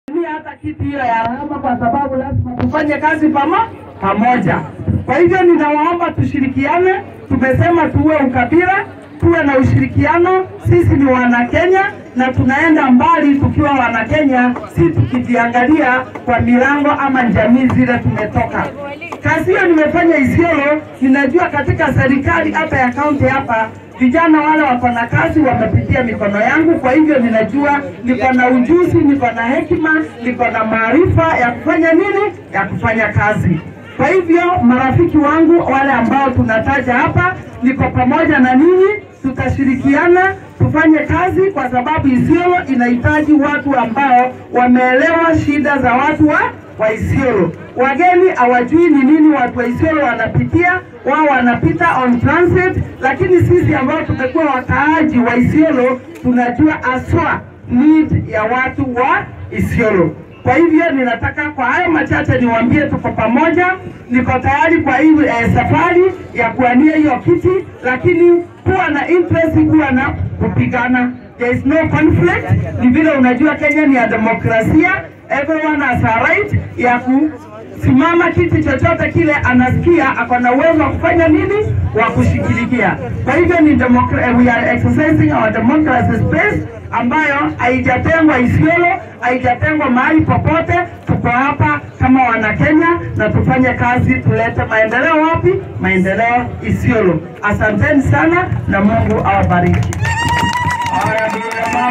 Another aspirant joins race for women representative position
The race for Isiolo Women representative position has heightened following announcement of a new candidate vying on a Jubilee Party (JP) ticket.